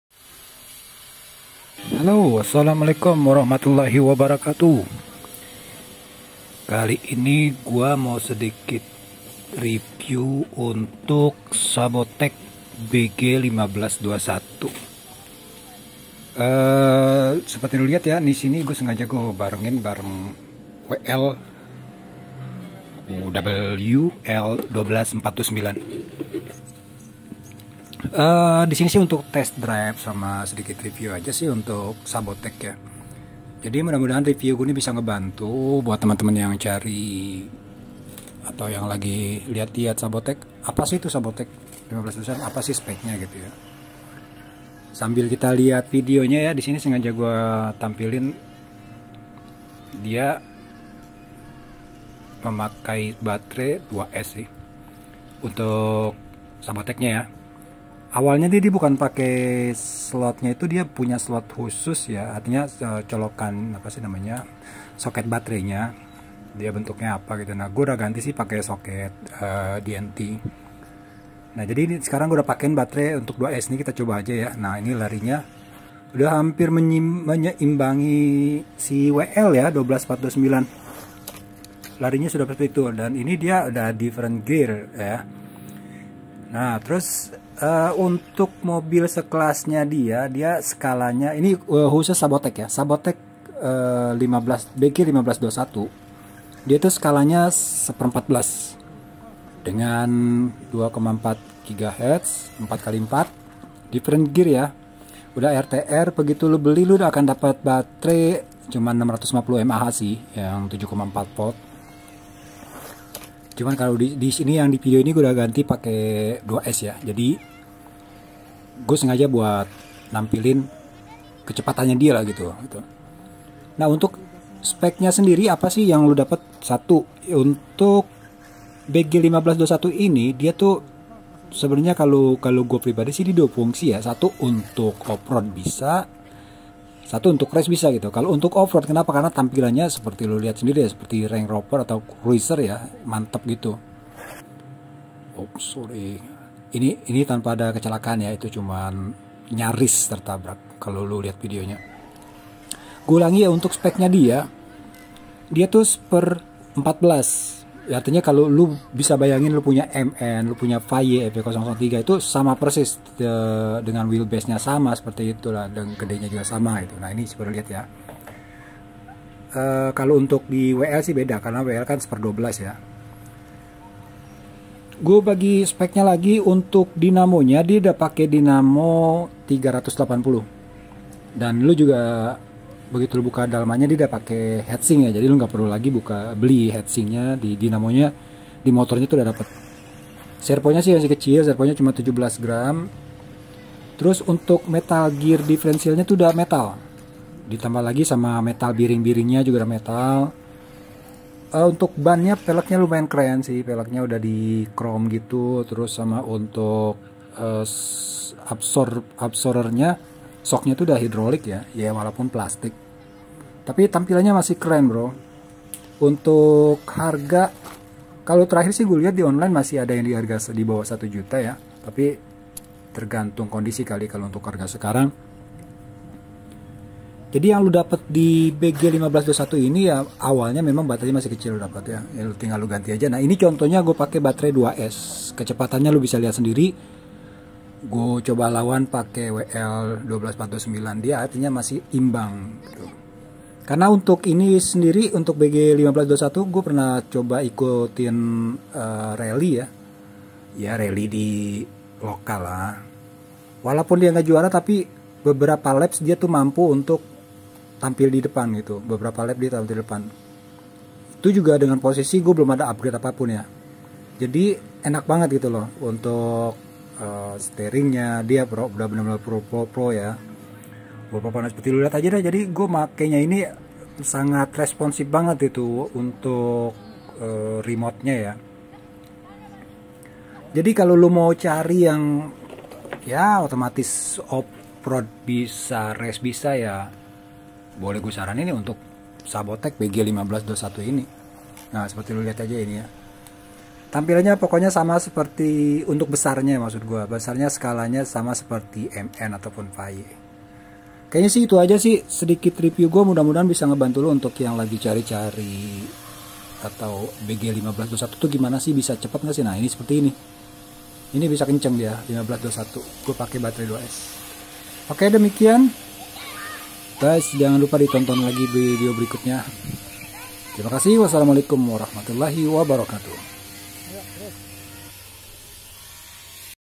Upgrade battrei 2s | test drive | Subotech BG1521 vs wl 12429
sesikit ulas balik subotech bg1521 vs wl 12429 semoga bisa membantu dan menjadi refferensi, terima kasih sudah menyaksikan dan nantikan video lainnya